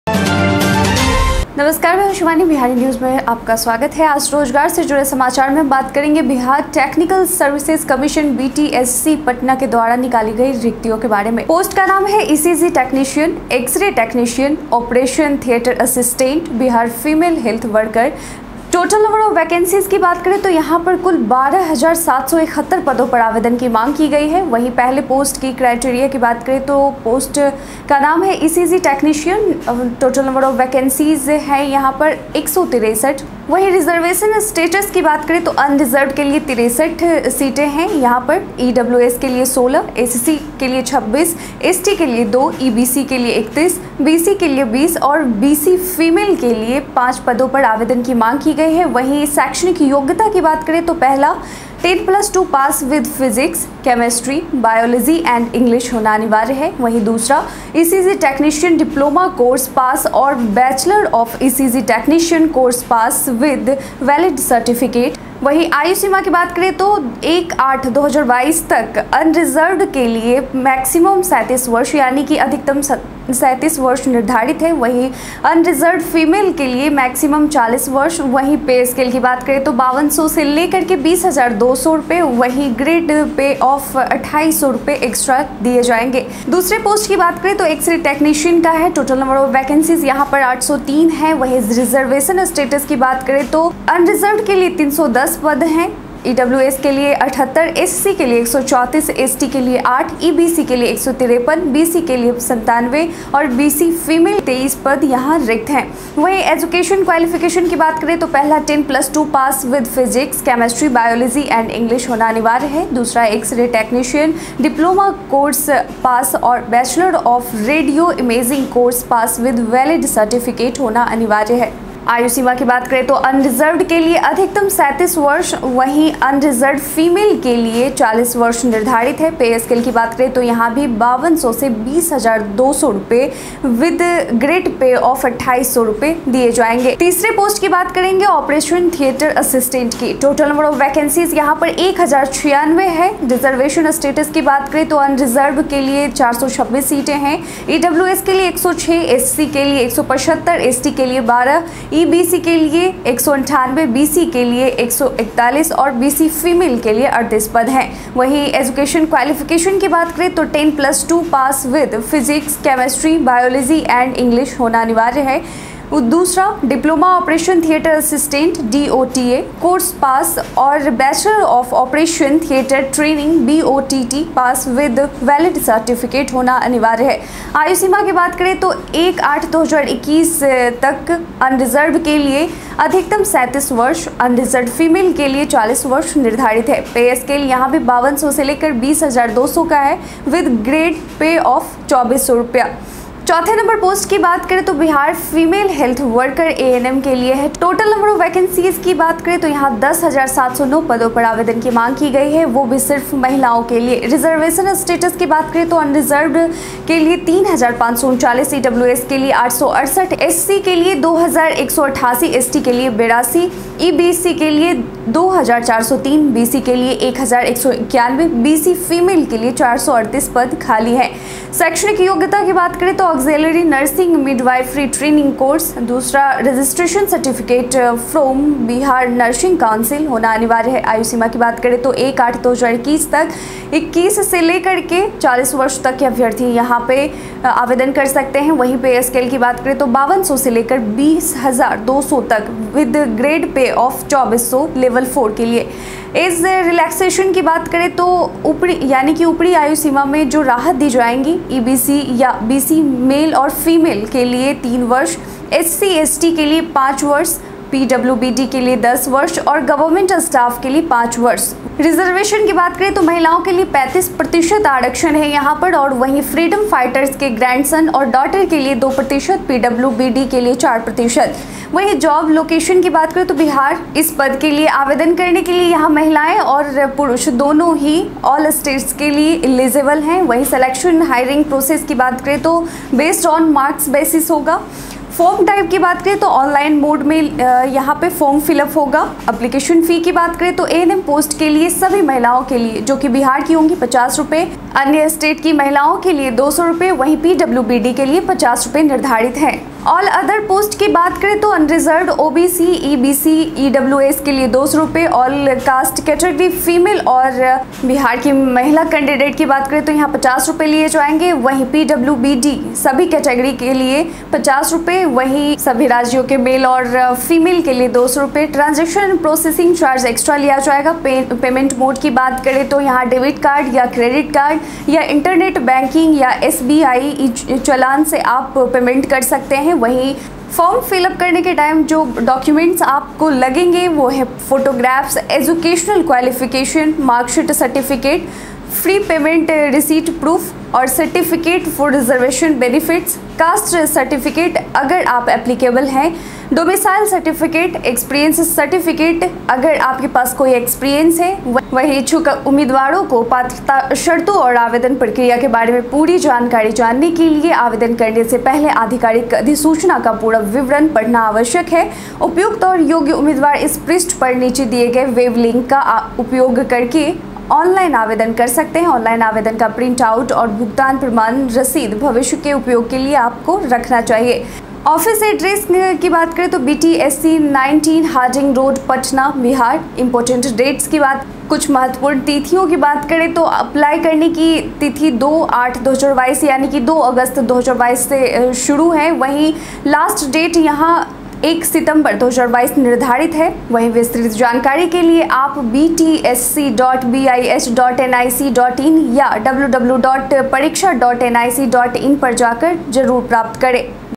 BTSC Government Jobs 2022 Apply for 12771 ECG & X Ray Technician, OT Assistant and ANM Vacancies
बिहार तकनीकी सेवा आयोग ने 12771 विभिन्न रिक्त पदों के पर भर्ती  लिए निकाली  रिक्ति विज्ञापन

To Know Full details of Vacancy Advertisement & Apply Online  -

Hash Tags -
BTSCANMFemale HealthWorkJobNotice